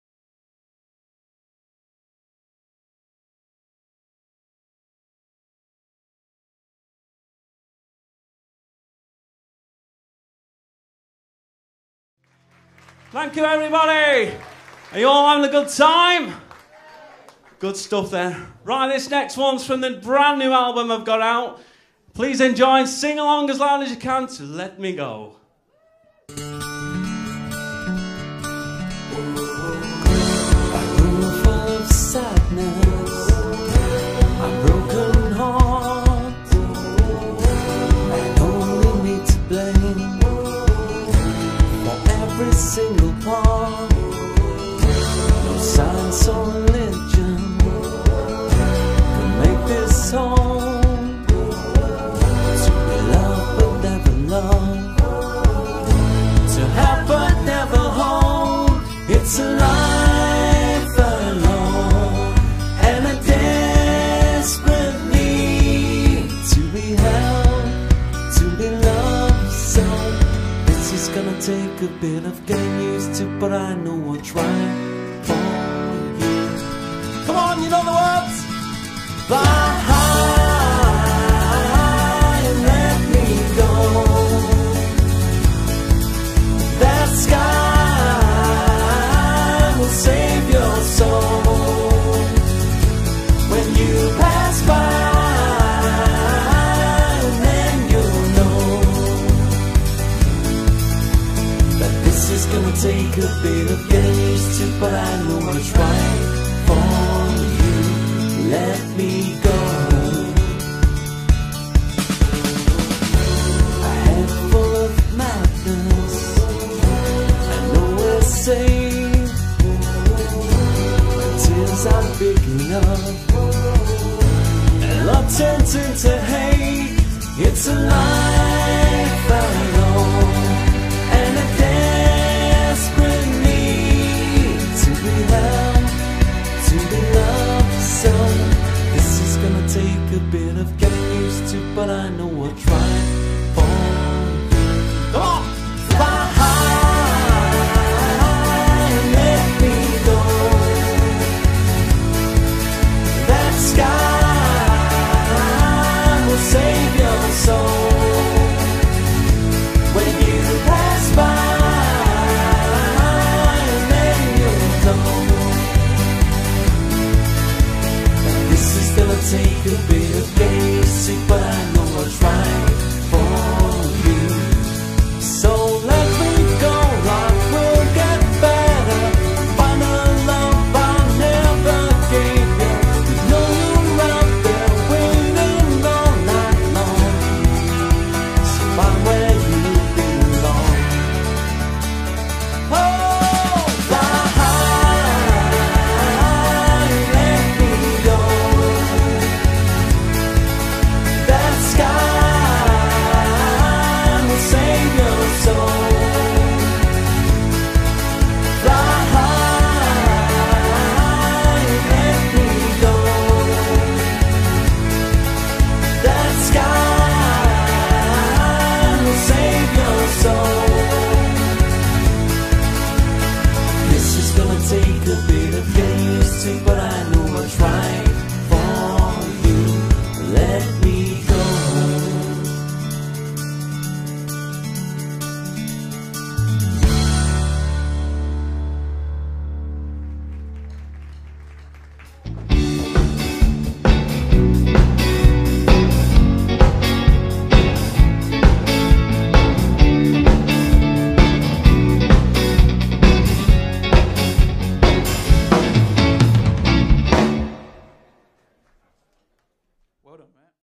Mark As Barlow | Gary Barlow Tribute Act | Big Foot Events
The most natural sound alike tribute to Gary Barlow in the UK. Mark has the same characteristic Manchester tone which lends itself to and incredible vocal and a dead ringer for the real Gary Barlow.